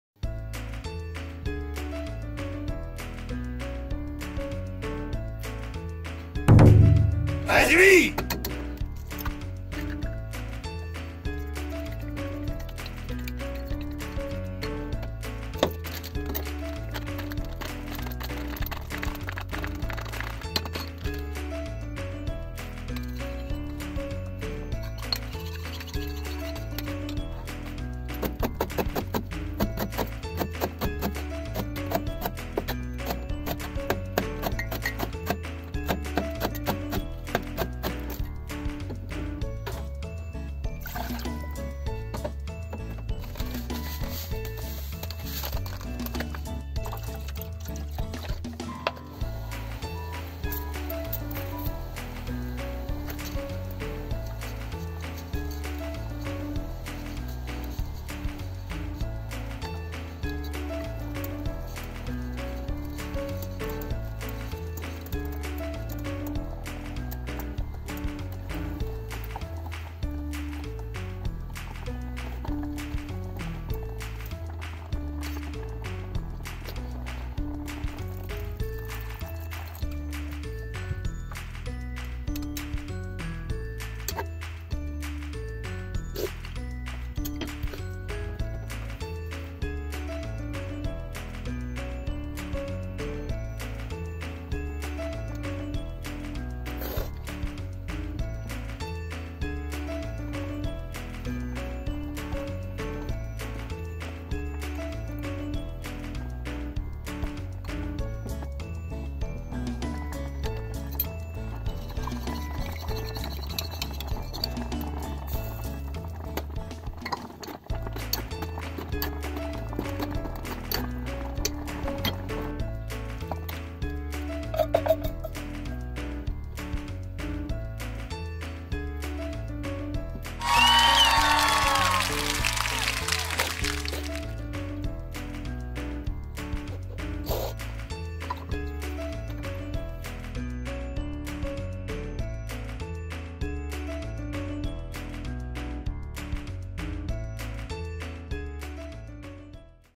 【ズボラ飯】ひやむぎの乾麺の塩気をそのままツユにいかしてかき玉のにゅうめんを作ります
夏には茹でて水でしめてツルッといただいてたそうめんやひやむぎもこれから寒くなるとにゅうめんにすることが多くなりますね
乾麺の塩気はそのままツユにできますね